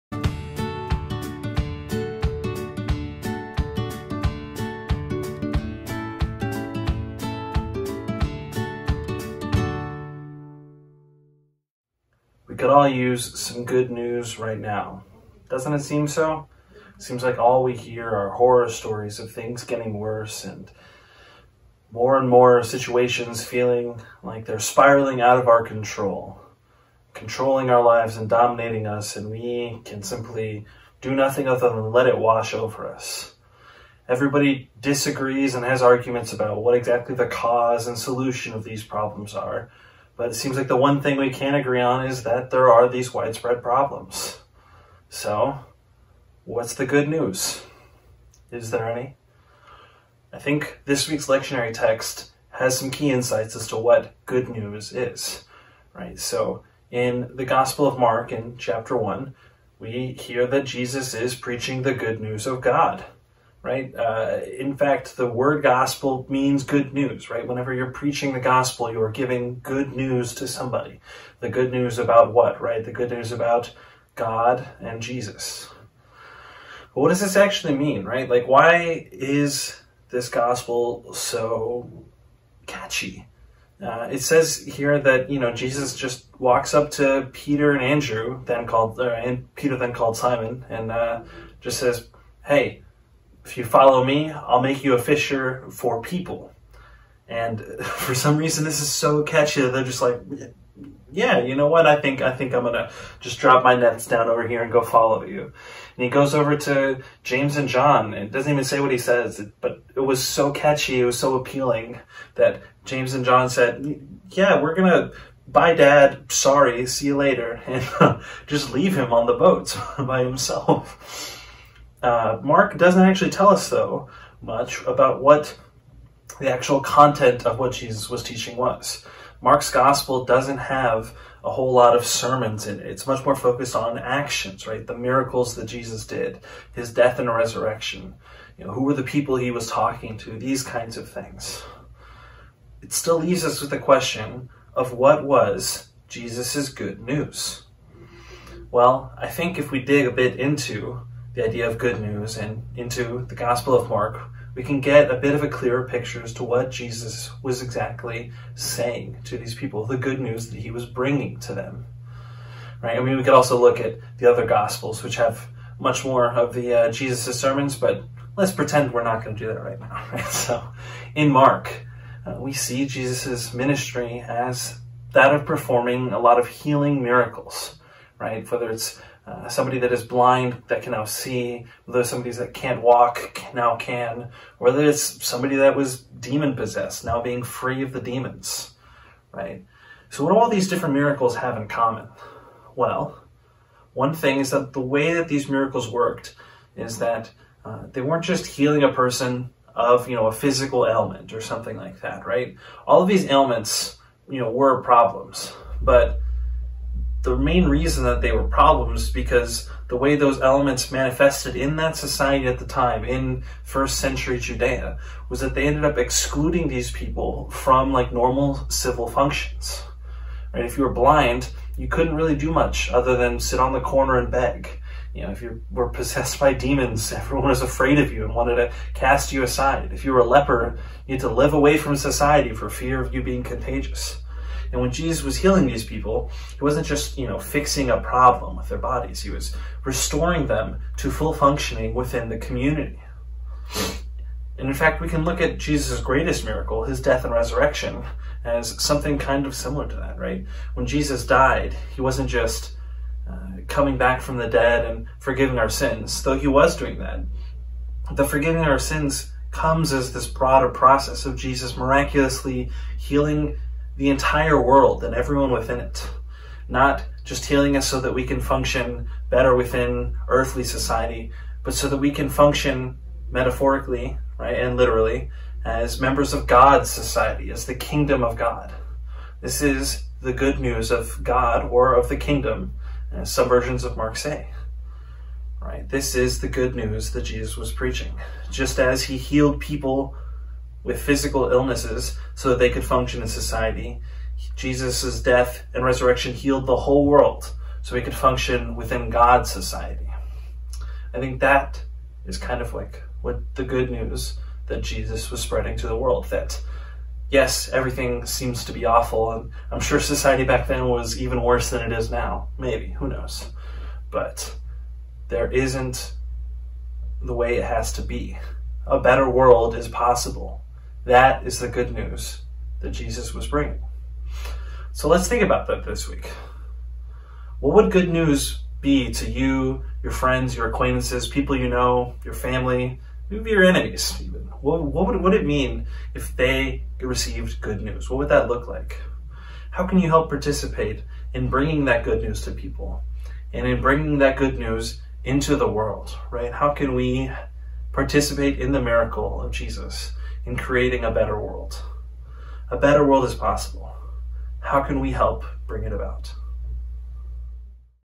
AHH: What is the Good News?
Stephen shares his thoughts on what is the Good News.